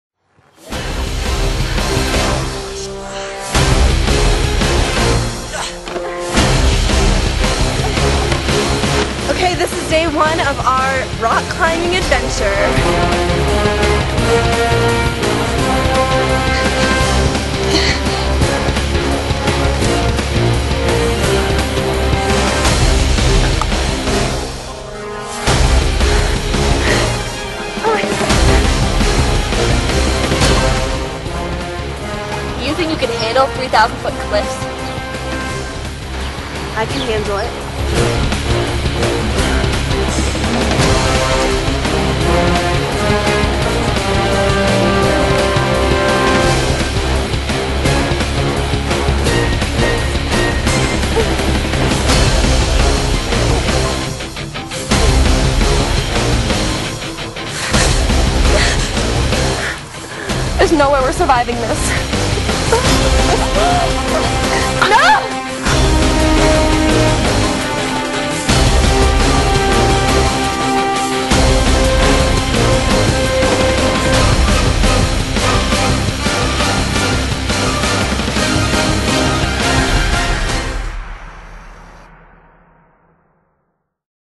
Climbing Catastrophe
Two clearly experienced and talented rock climbers embark on an adventure that goes extremely wrong.  (Movie trailer)

Sorry about the poor quality, but until I can get a better program... This is mostly just for Sage! Top thoughts :)